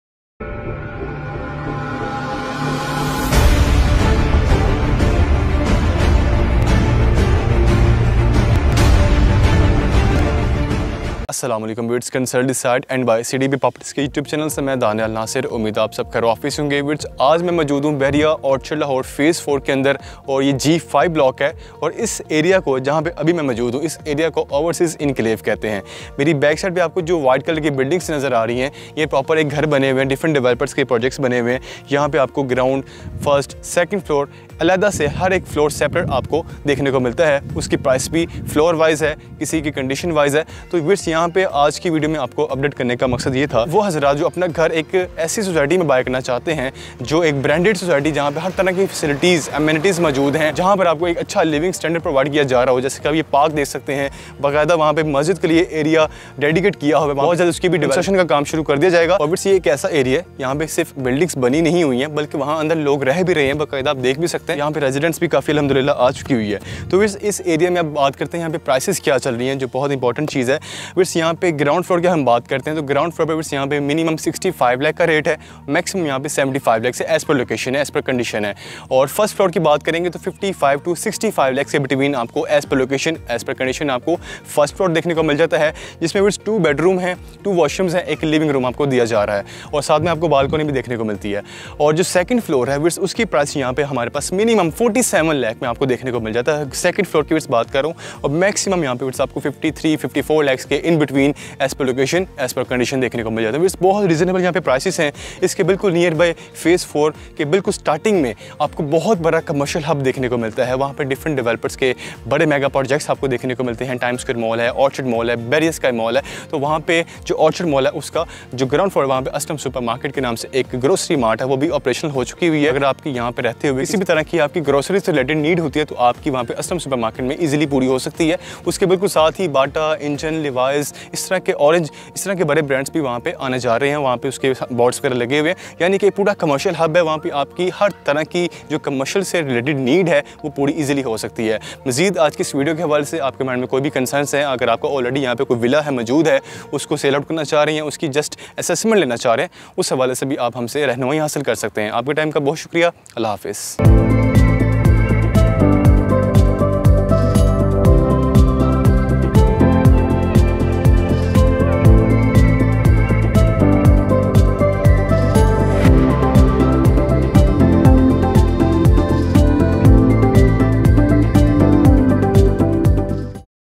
Independent Separate House in Bahria | 5 Marla House, Price 50 Lacs | July 2024
• Jamia Masjid
• Community Parks and Zoo
• Commercial Areas

↪ District One
↪ Lahore Smart City
↪ Lake City
↪ Al-Kabir Town
↪ Alnoor Orchard
↪ New Lahore City
↪ Zaitoon City
↪ BEMC